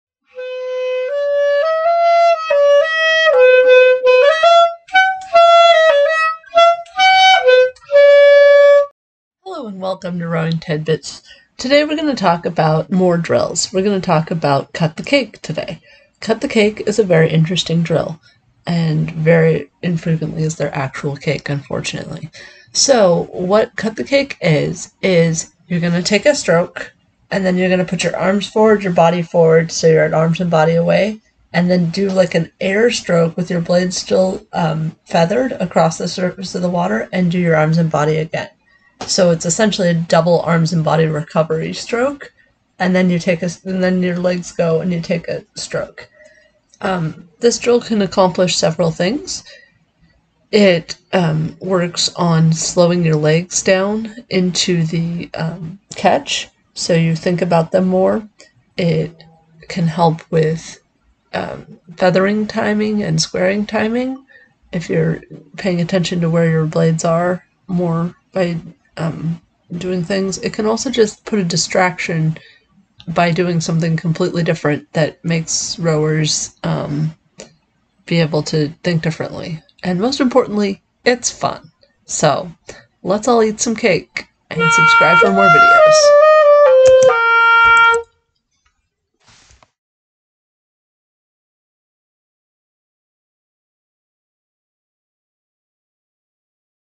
Cut the cake Rowing Drill
This video shows rowers how to do the cut the cake drill. It is good for novice rowers who are rushing their slide, can't get timing right, or just want to have some fun. It shows how to do the drill and why you do it.